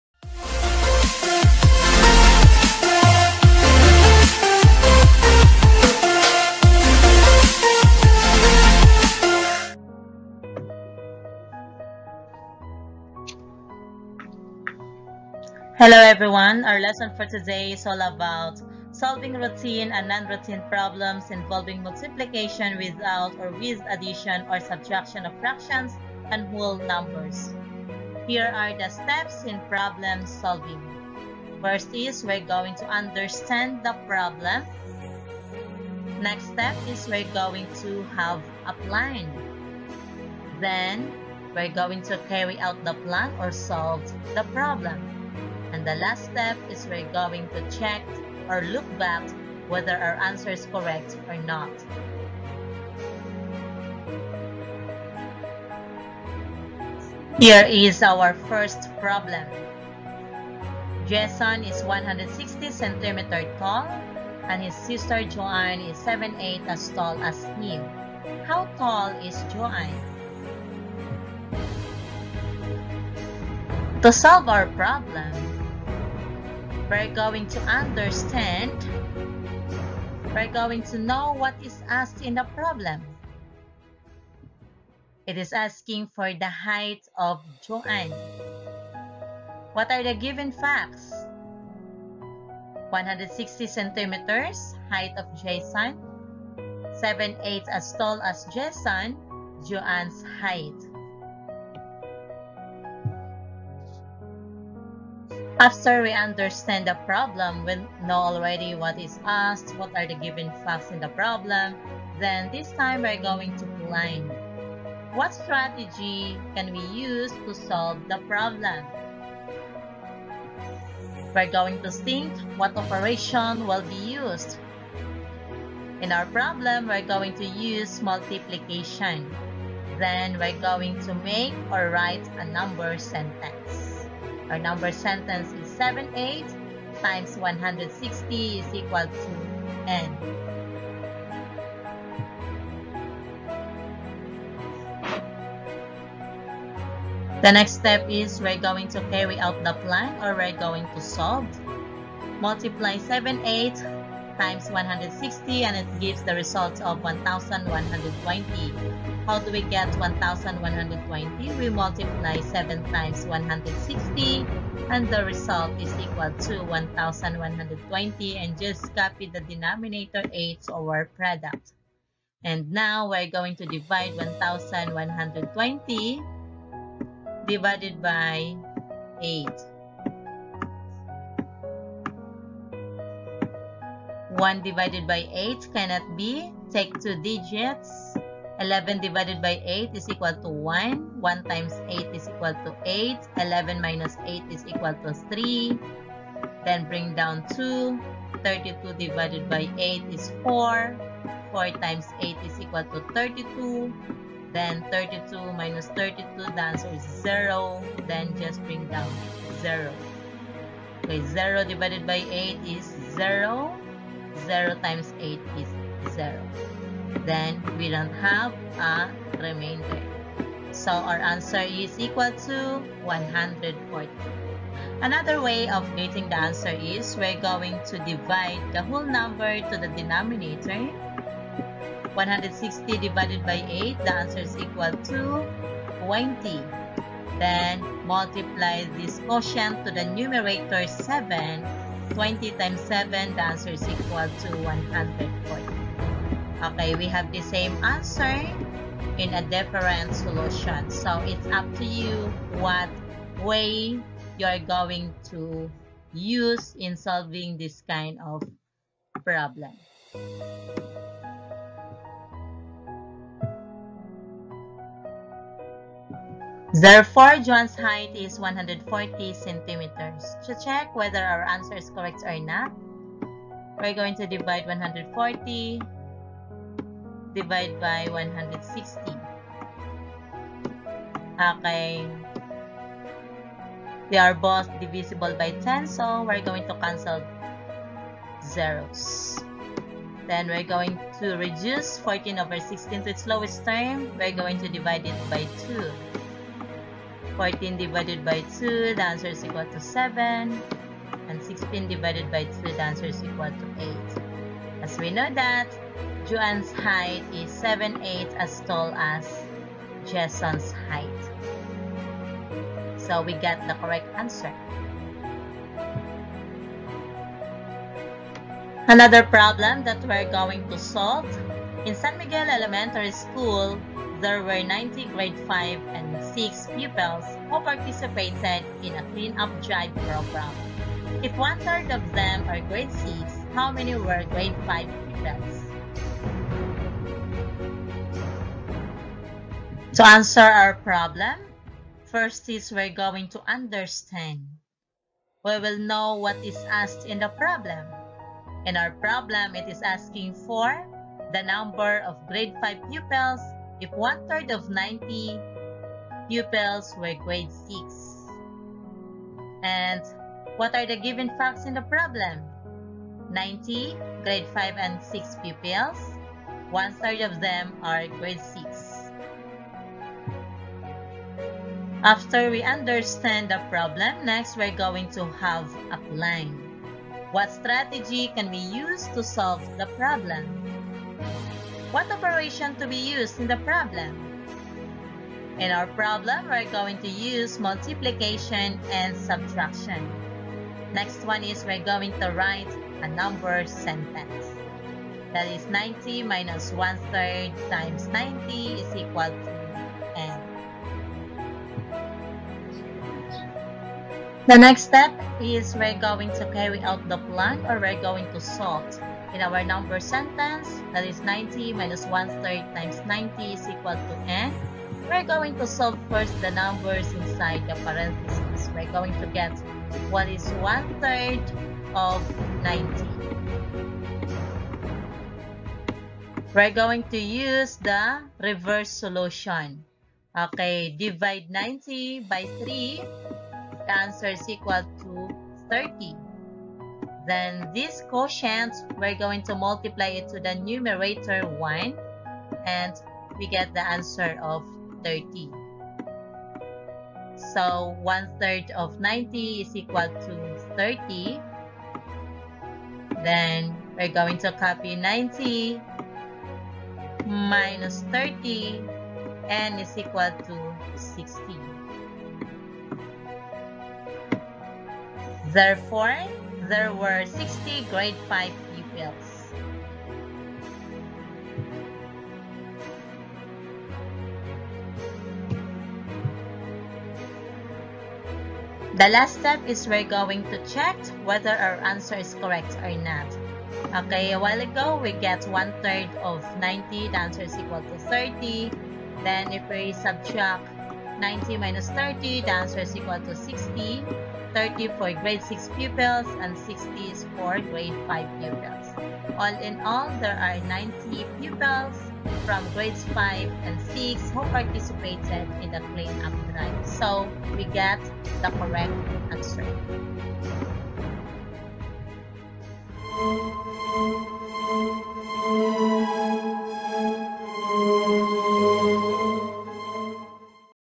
Solving Routine or Non- Routine Problems Involving Multiplication
This video is all about Solving Routine or Non- Routine Problems Involving Multiplication without or with Addition or Subtraction of Fractions and Whole Numbers